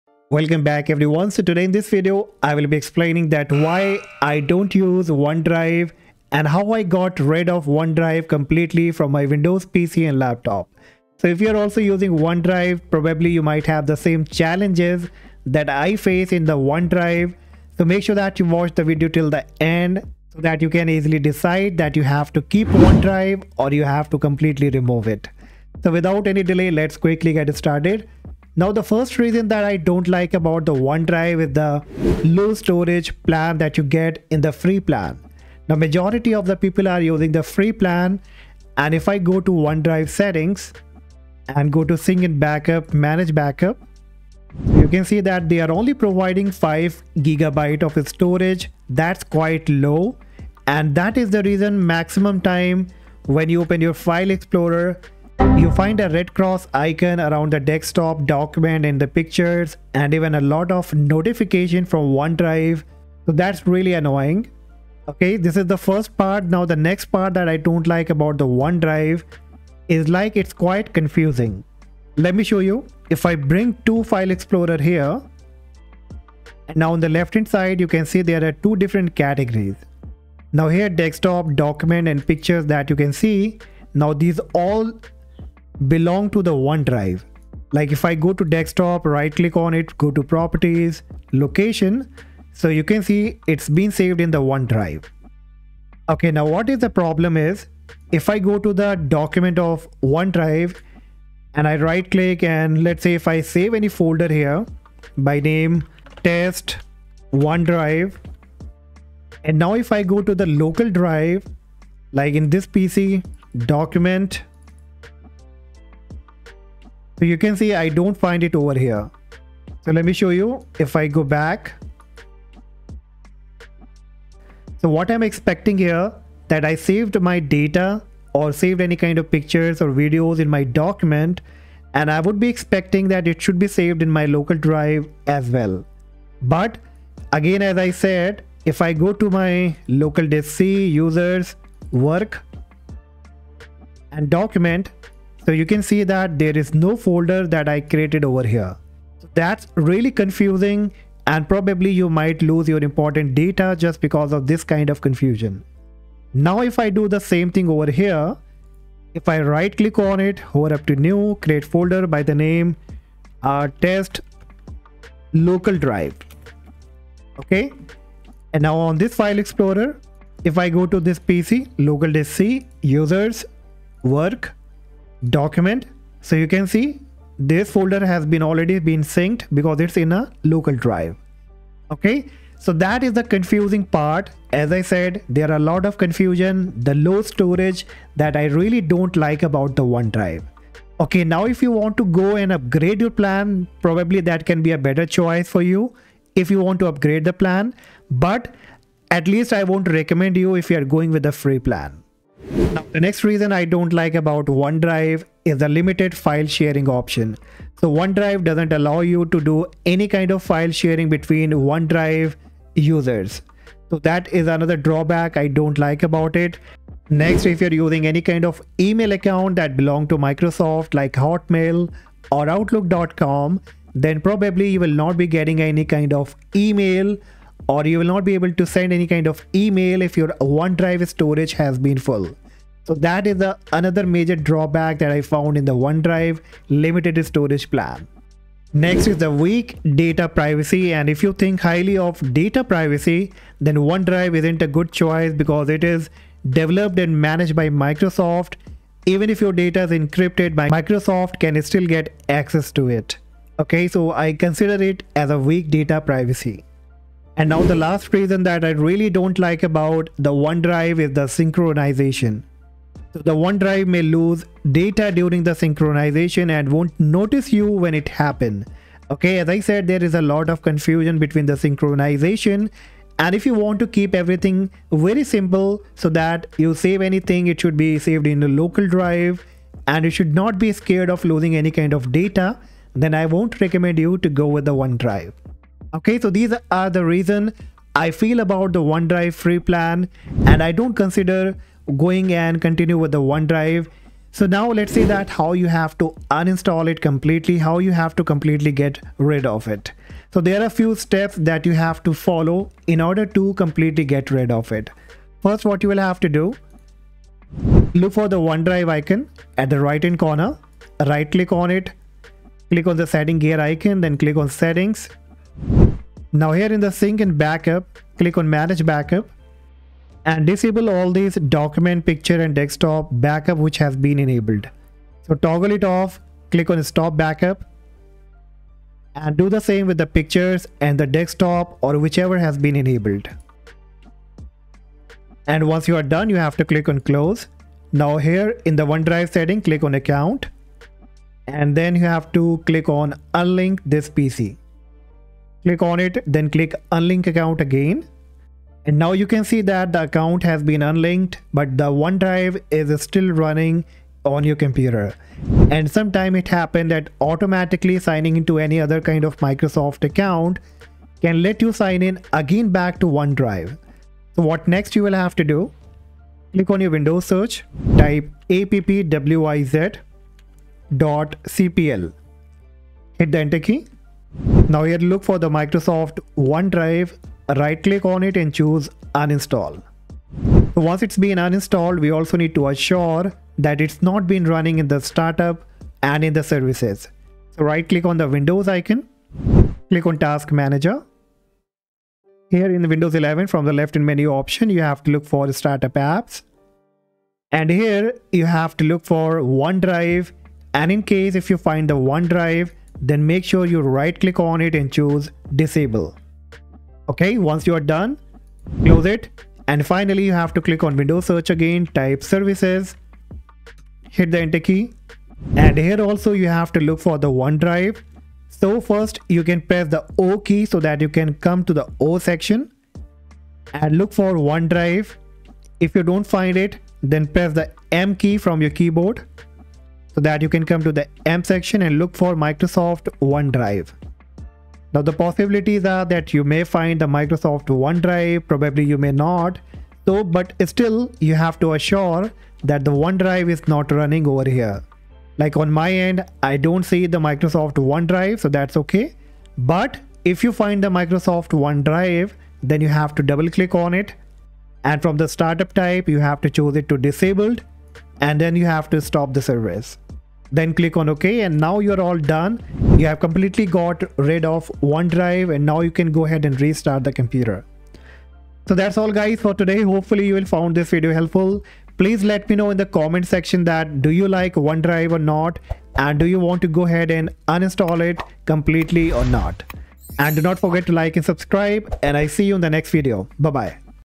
❌Why I Don't Use ONE DRIVE? ❌| How to Get RID of ONE DRIVE
There are several reasons i stopped using one drive and that is what I will be sharing with you in this video and also explain how to completely remove one drive from your Windows 10 and 11.

☑ Watched the video!
☐ Liked?